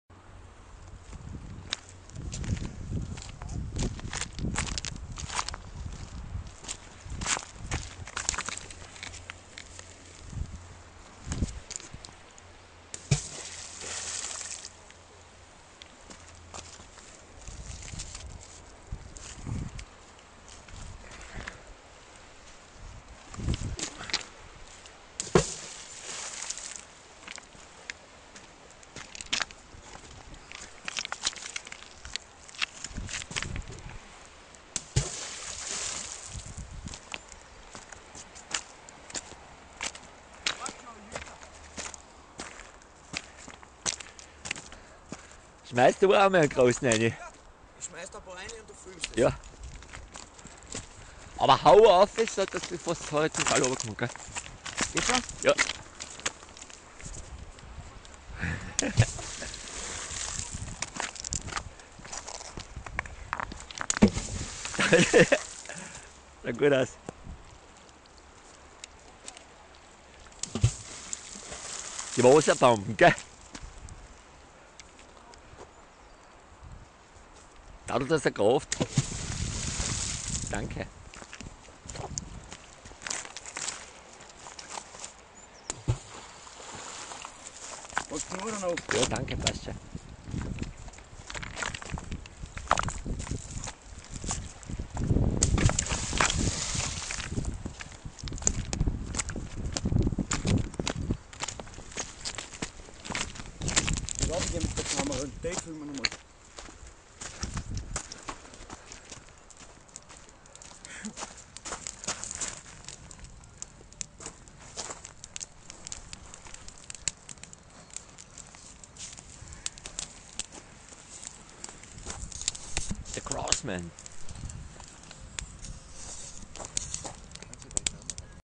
Wasserbomben werfen
Jugendliche tapfere Burschen der besten genetischen Qualität und Güteklasse, die ehrfürchtig der Gottheit gegenübertreten und Life and Nature in die heimische Flusslandschaft einbringen, sollen nicht kriminalisiert und diskriminiert werden, sonder gelobt und honoriert werden im Namen der himmlischen Hoheit für ihre würdevolle Tapferkeit im taufrischen Knabelalter